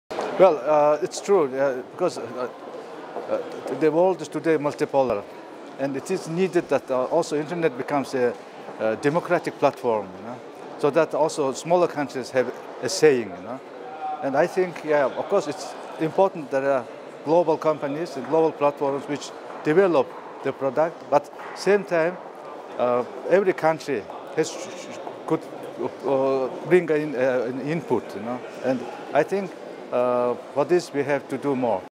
Global IGF 2019: Bold Luvsanvandan on Global Internet Platforms
Bold Luvsanvandan, member, The State Great Hural (Parliament) of Mongolia, Mongolia, shares their answer to the question, “Some people say the concentrated power of global internet platform companies like Google, Facebook, Tencent and Baidu is dangerous. Why is this seen as a threat and what can be done about it?” Recorded at the Global Internet Governance Forum 2019 in Berlin, Germany, by the Imagining the Internet Center, an initiative of Elon University, North Carolina, USA.